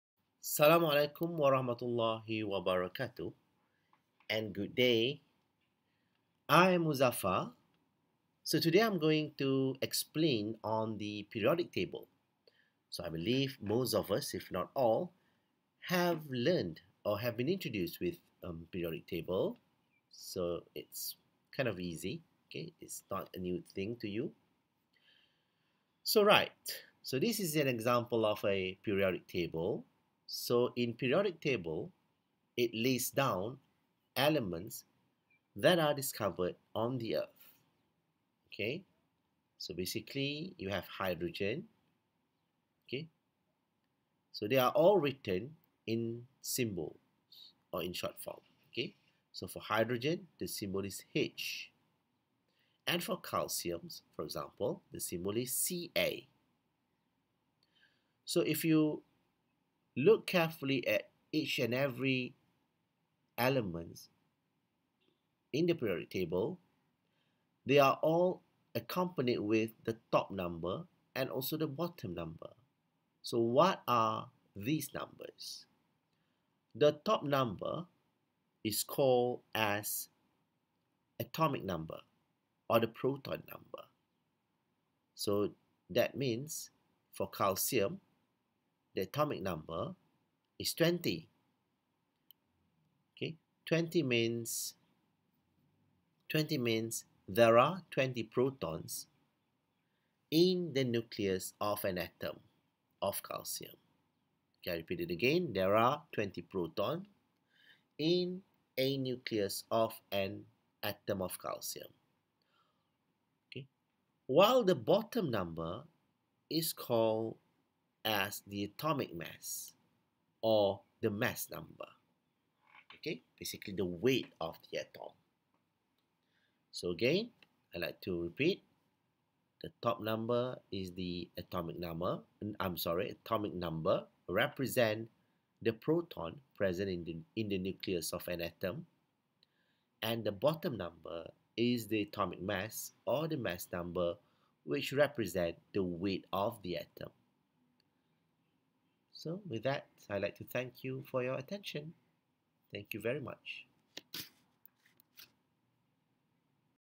2.0 Periodic Table
Introduction to Periodic Table, Element, Atomic Number and Atomic Mass